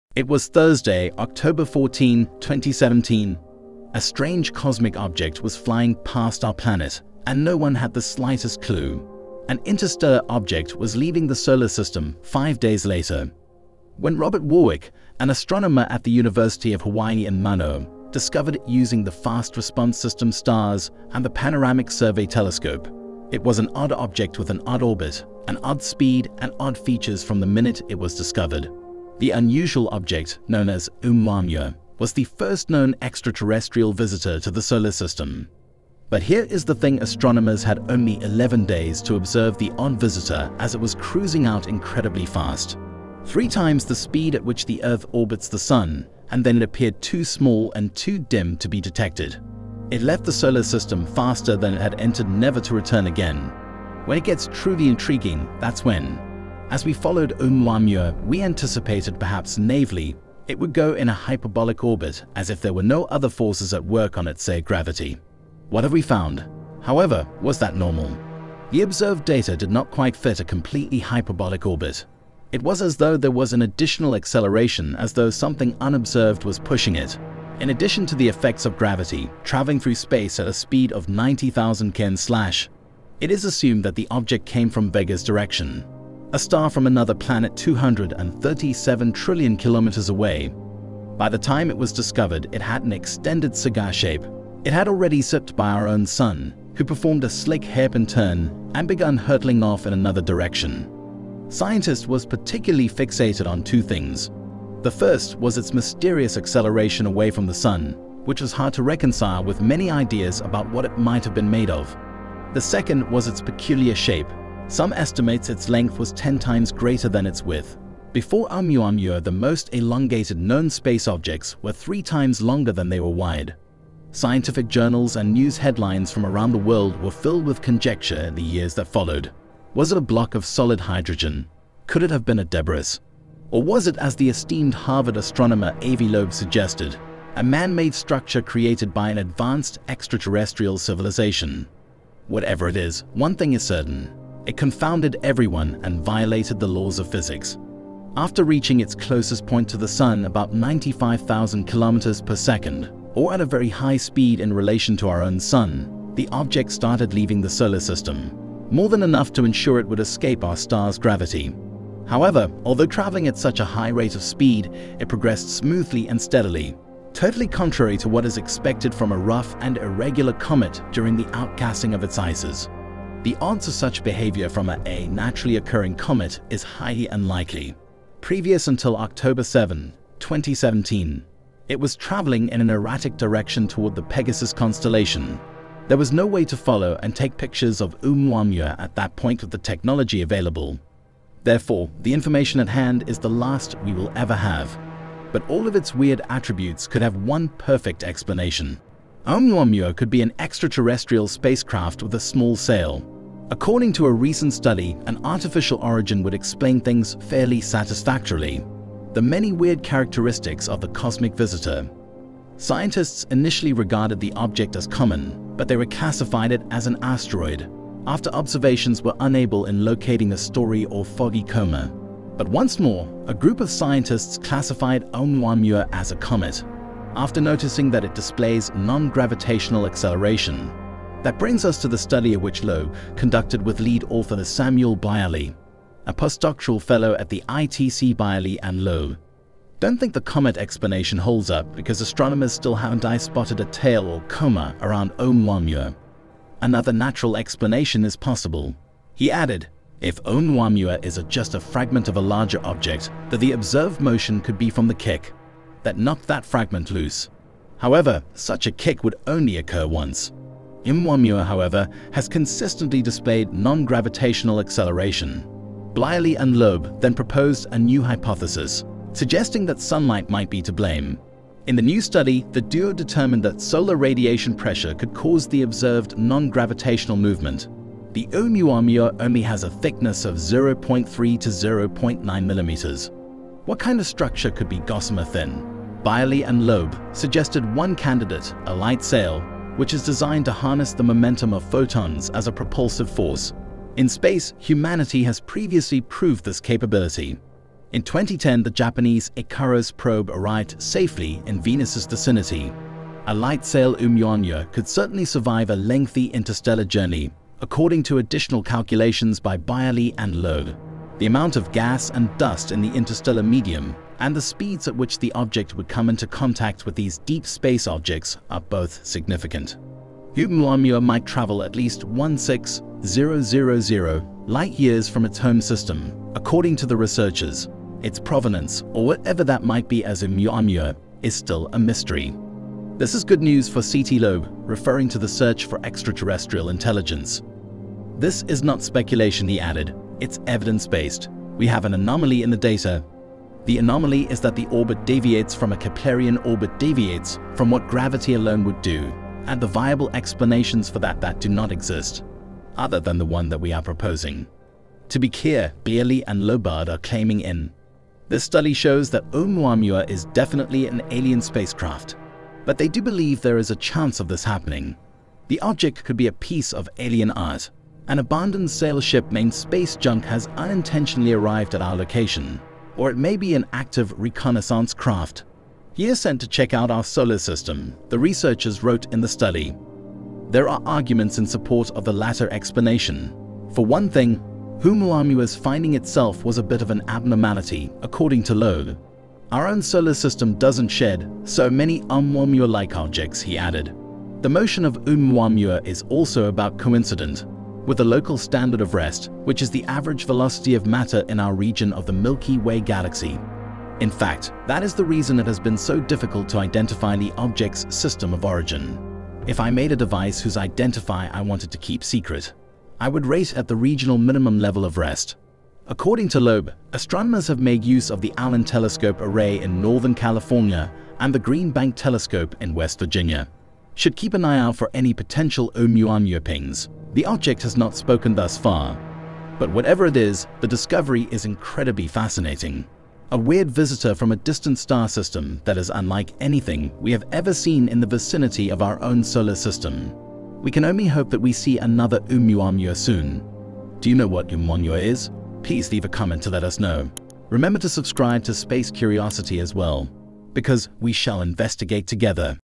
Massive Terrifying- Unidentified Objects 300 Miles Wide Have Been Detected By NASA
It was Thursday, October 14, 2017.
A strange cosmic object was flying past our planet, and no one had the slightest clue.
An interstellar object was leaving the solar system five days later.
When Robert Warwick, an astronomer at The University of Hawaii in Manoa, discovered it using the fast response system Stars and the panoramic survey telescope. It was an odd object with an odd orbit, an odd speed, and odd features from the minute it was discovered.
The unusual object, known as Oumuamua, was the first known extraterrestrial visitor to the solar system.
But here is the thing astronomers had only 11 days to observe the odd visitor as it was cruising out incredibly fast.
Three times the speed at which the Earth orbits the Sun and then it appeared too small and too dim to be detected.